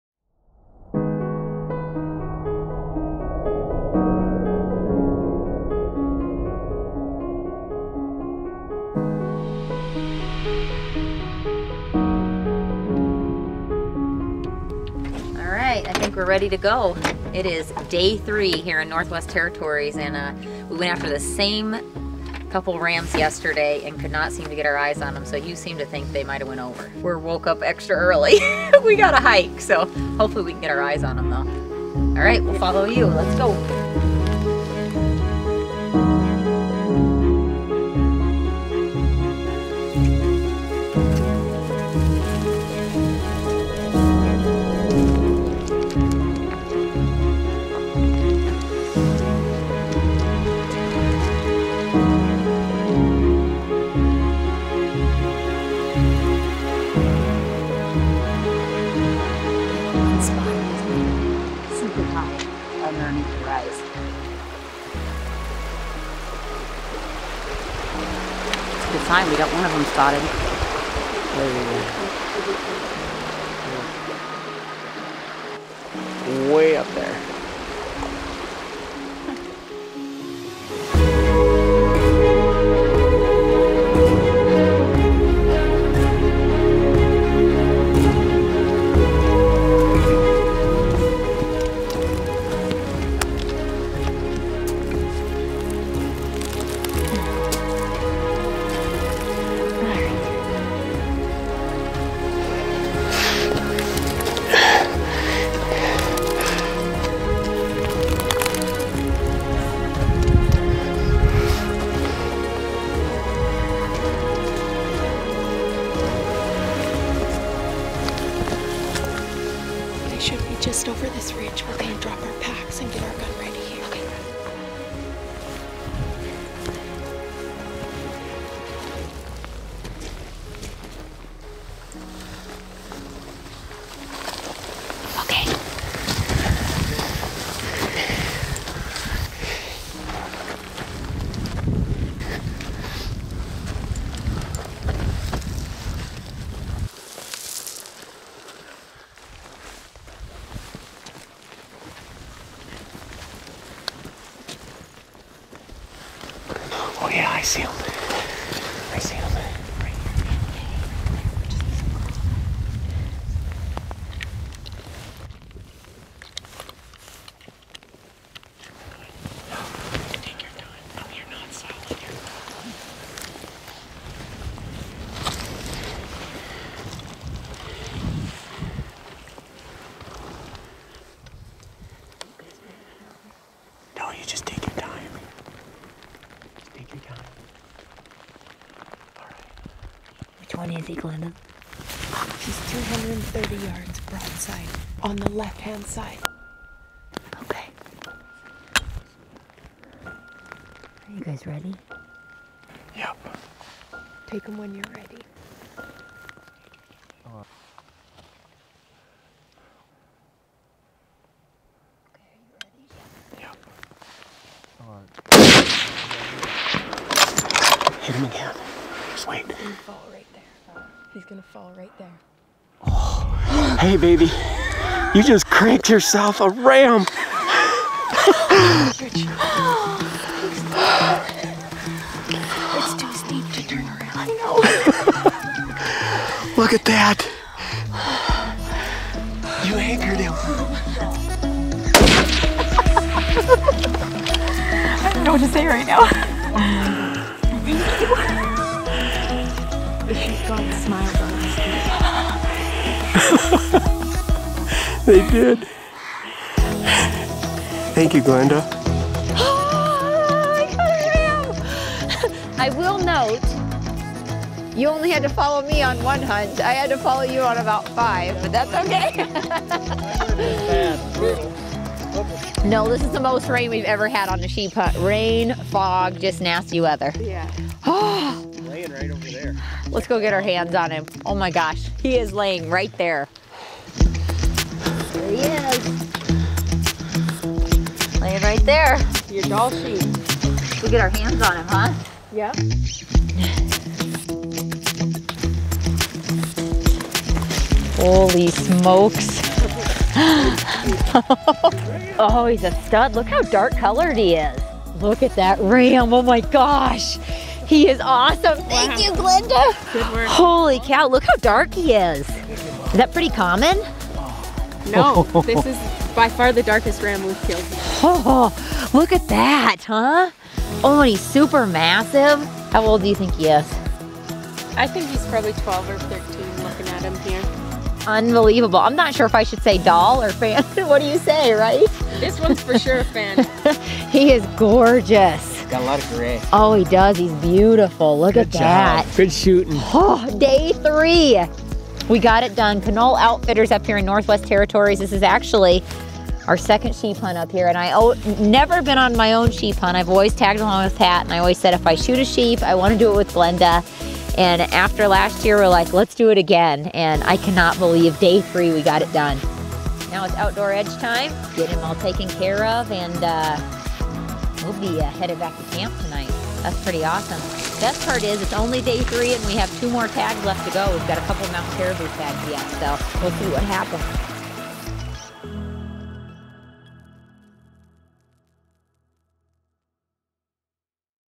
My First Sheep Hunt | Dall Sheep in the Northwest Territories
Nicole is hunting Dall sheep at Canol Outfitters in the Northwest Territories when they spot two mature rams at the top of the mountain.